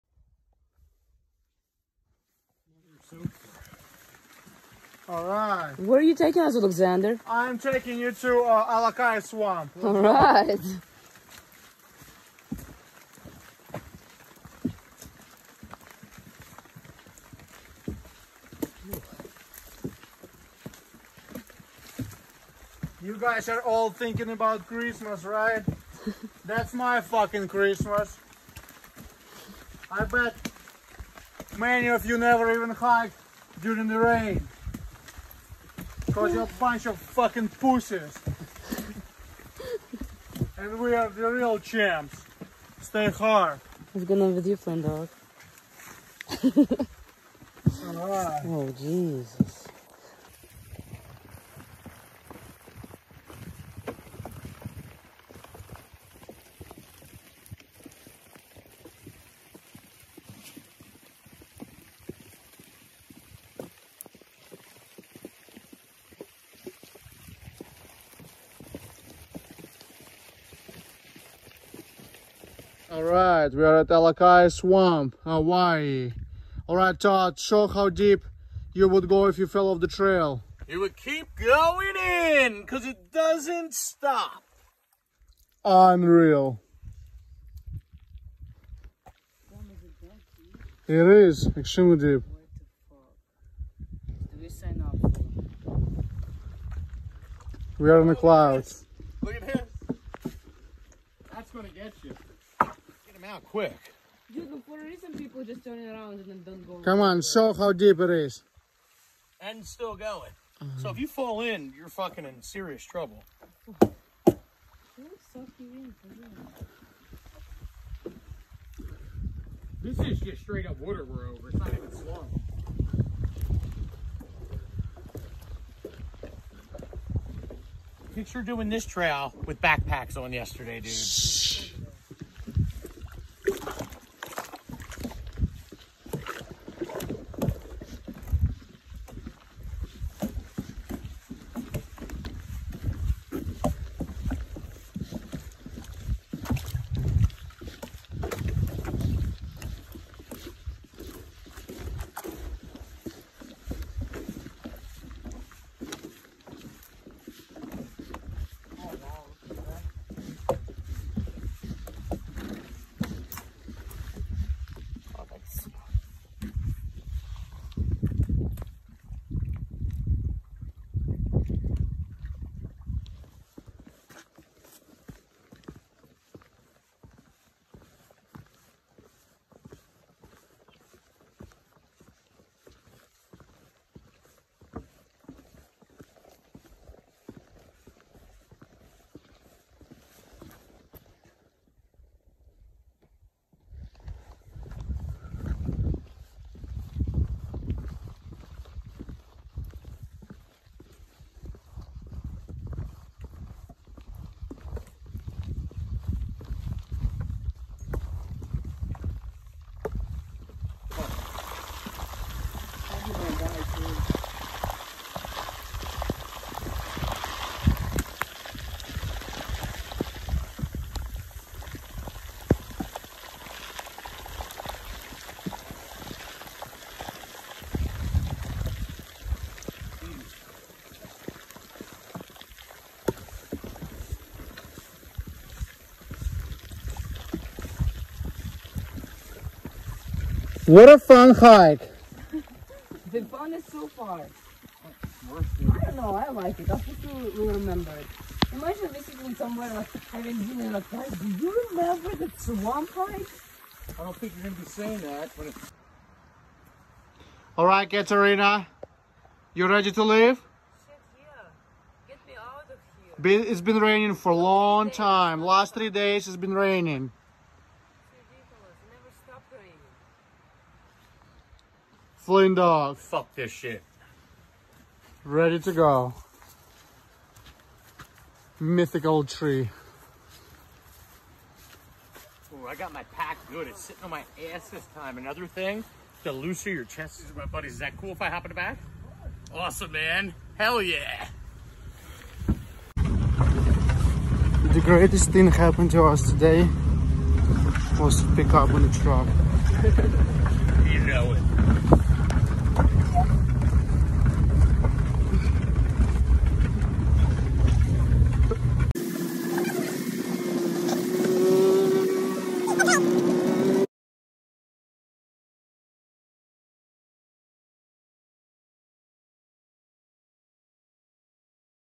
ALAKAI SWAMP TRAIL. Kauai, HI 🌈⛰🤙
If you looking for something challenging and unique this would be a great hike. Located at kokee state park area. You can hike up and camp there or you could drive up to camp grounds and get right to the hike. But make sure you take a truck or a car that is able to through the mud and big ass potholes🙂 if you hike it’s extremely wet bring a good waterproof tent ⛺️ , waterproof hiking shoes will be really appreciated.